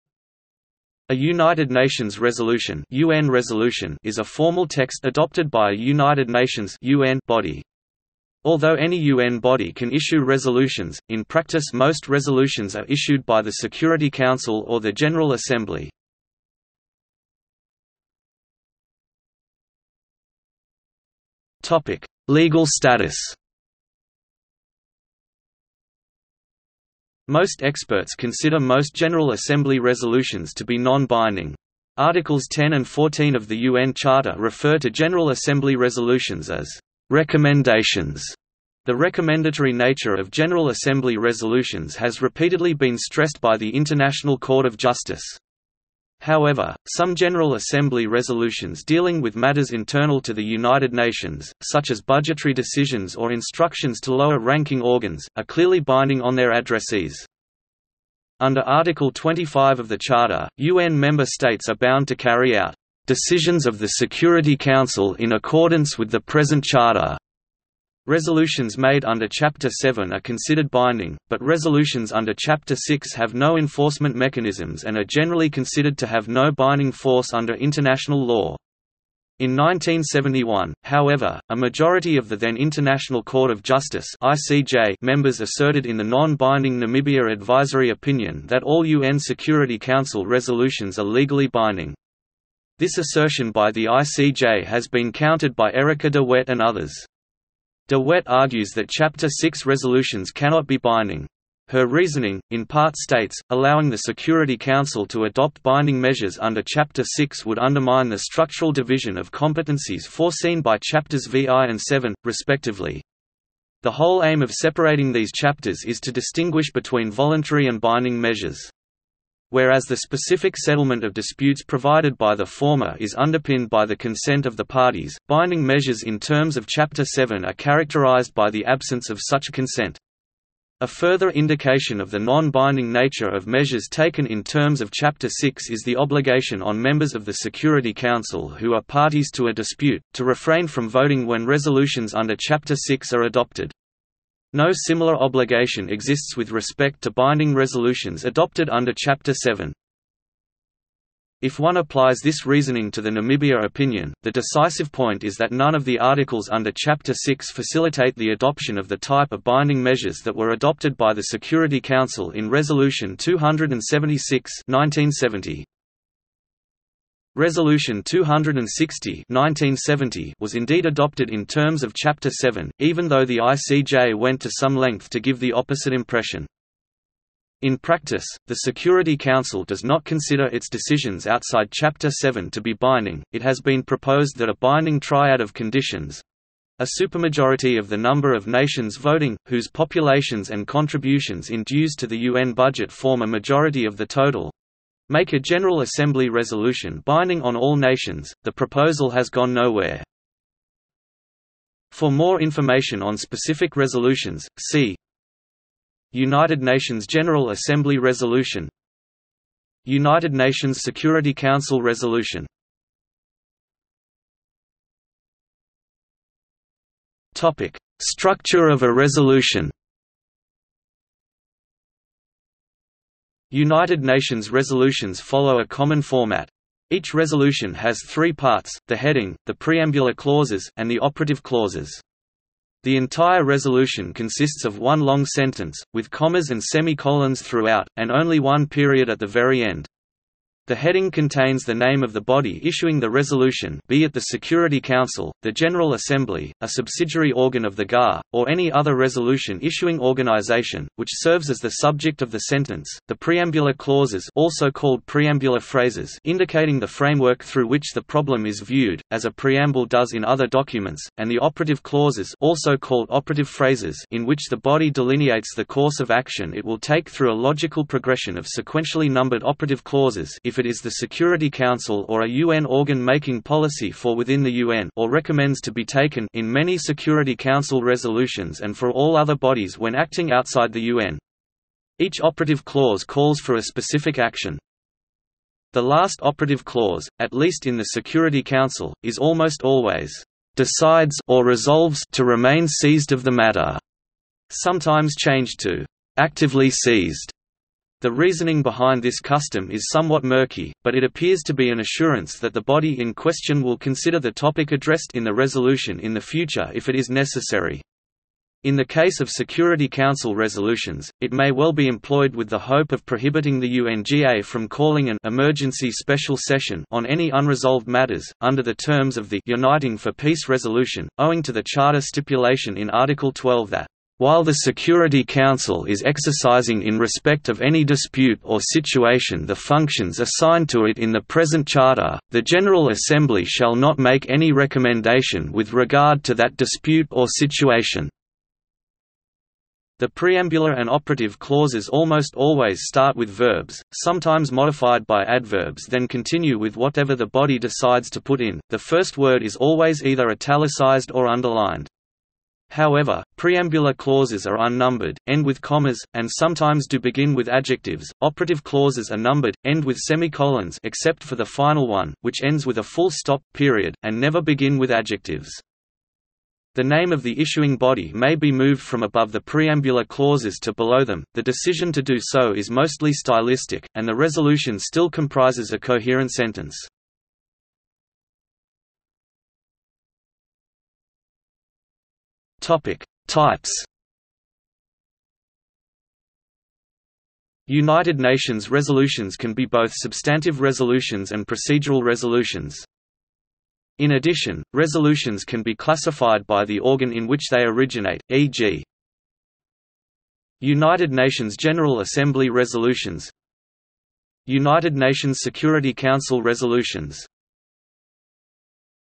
United Nations resolution | Wikipedia audio article
00:00:17 1 Legal status
00:03:33 2 Structure of a resolution
00:06:18 3 Types

- Socrates

SUMMARY
A United Nations resolution (UN resolution) is a formal text adopted by a United Nations (UN) body. Although any UN body can issue resolutions, in practice most resolutions are issued by the Security Council or the General Assembly.